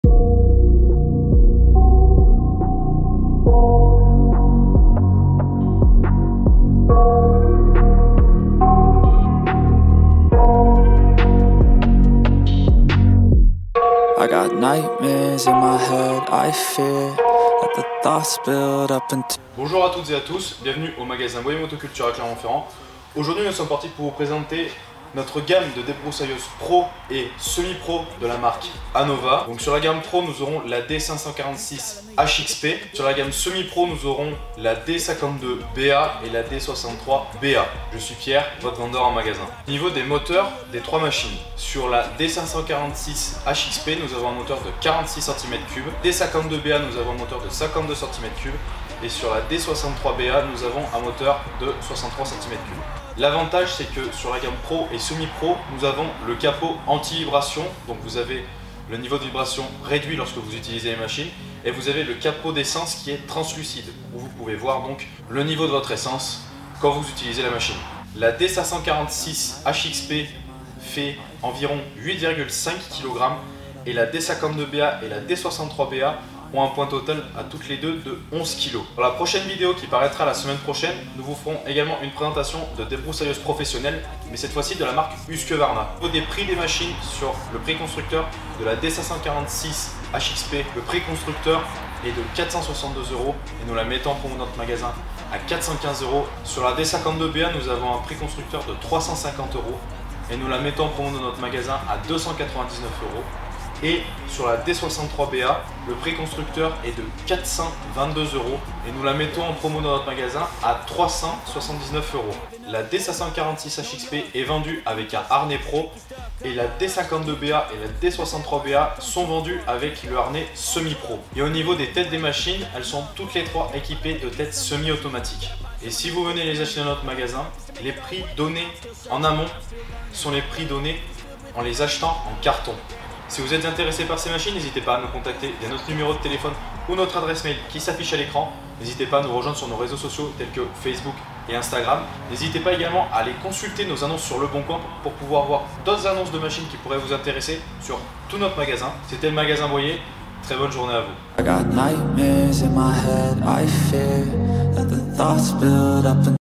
Nos débroussailleuses professionnels Anova - Présentation - Boyer Motoculture
L'entreprise Boyer Motoculture vous présente sa gamme de debroussailleuses professionnelles Anova

D546 HXP, D52BA, D63BA

Moteur : 46cc, 52cc, 63cc
Poids : 8,5kg, 11kg, 11kg

Réduction des vibrations
Réservoir translucide

Vous êtes intéressé par ce matériel ?

Concession Husqvarna Snapper Etesia Staub

BOYER MOTOCULTURE
41 AV DE LA LIBERATION/BOULEVARD JEAN JAURES
63000 CLERMONT FERRAND
A 800 METRES DU CENTRE JAUDE
PARKING CLIENTS